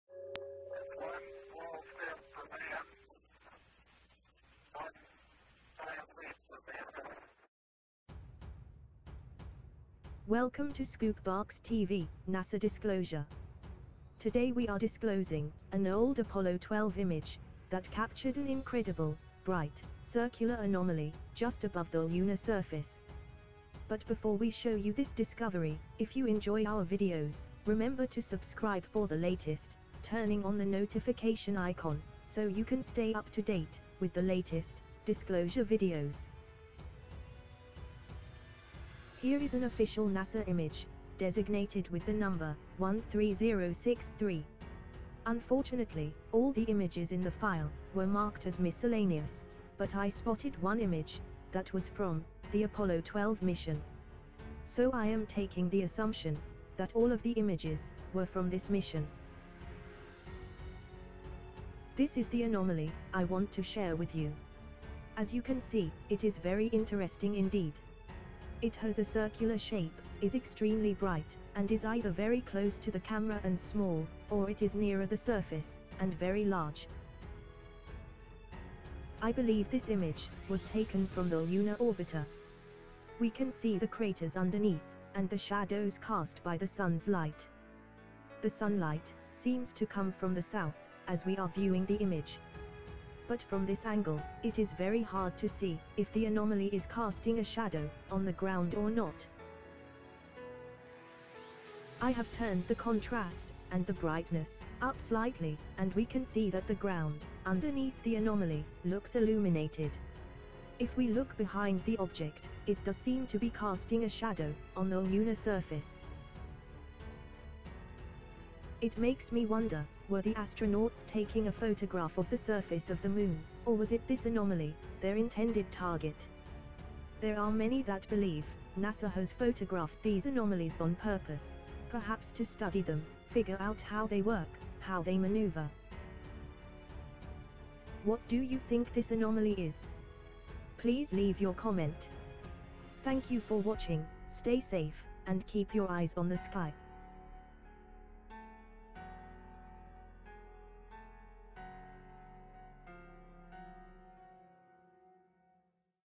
*NEW* NASA Won't show you THIS! Apollo12 Bright UFO Above the Moon?
Welcome to ScoopBoxTv NASA Disclosure!

Today we have a UFO Captured during the Apollo12 Mission to the moon!!